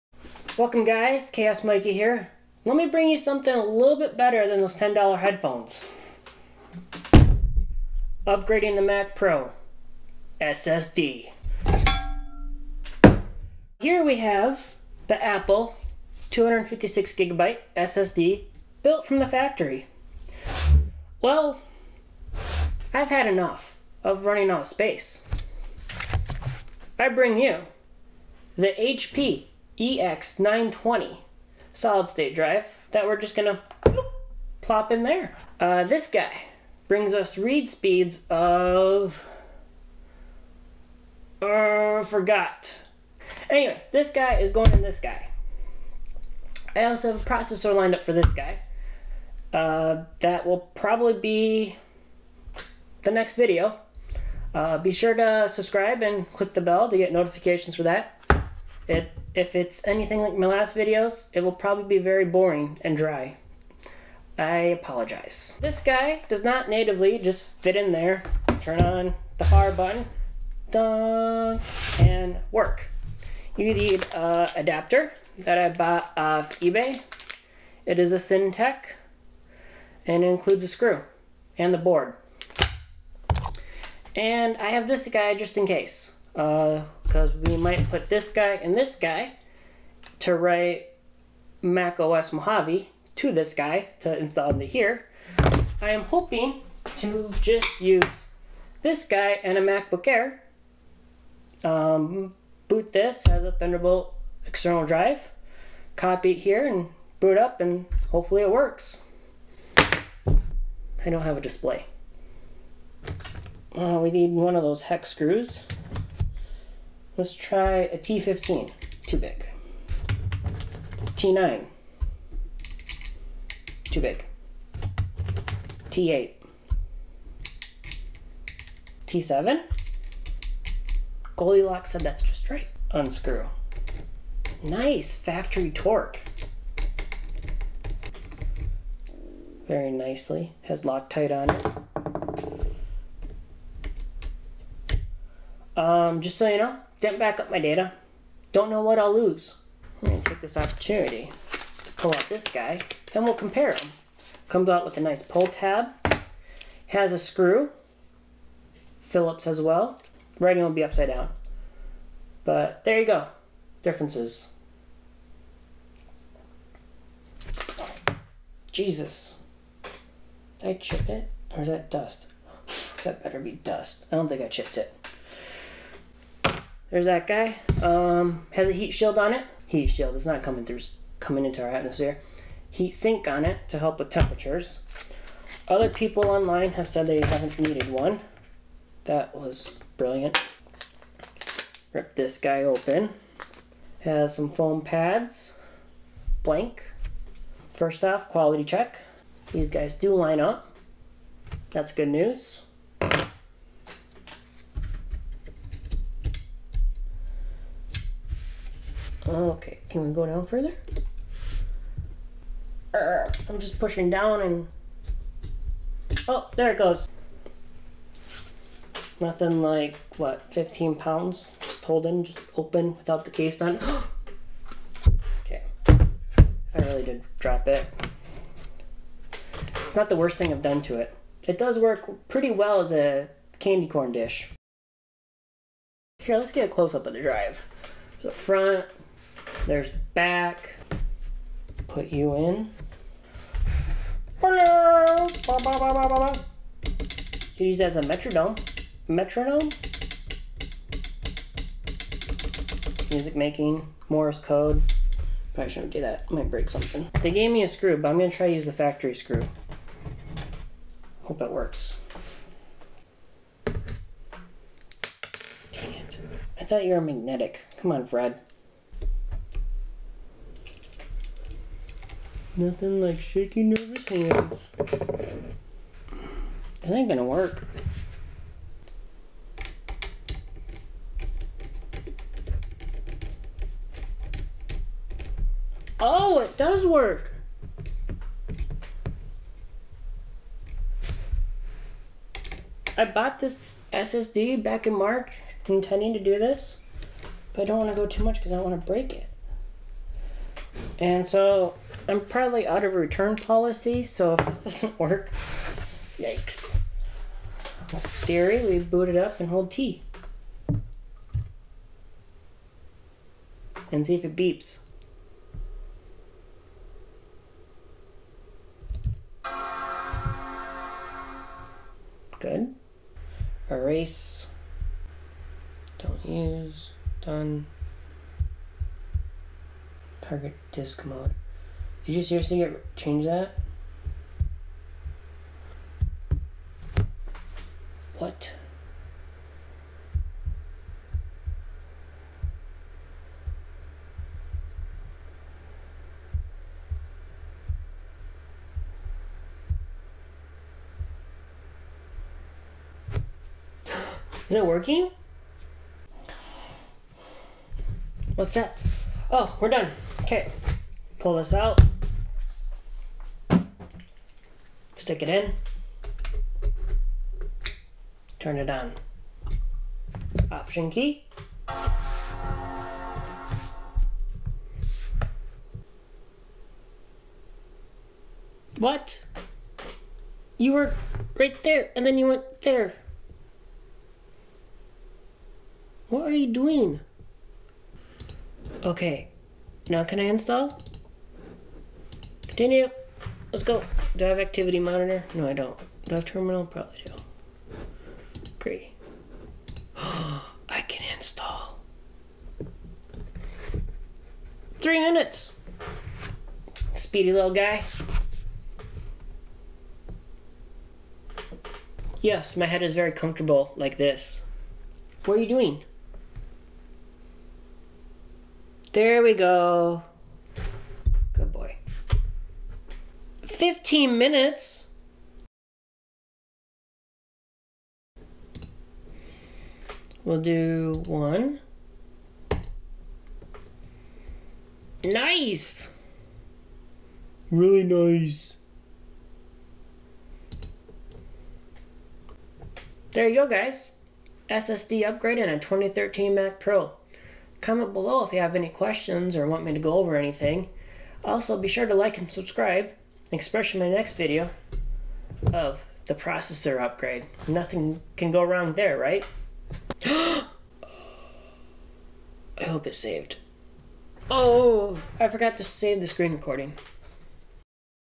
2013 Mac Pro SSD Upgrade with a HP EX920
I successfully attempt upgrading the 6,1 Mac Pro SSD with an HP EX920 1TB NVME Drive.

Sorry for the phone dying at the end of the video, I tried to salvage it. Someone forgot to plug the phone into power before recording. This person is again on a PIP and promised to do better next time.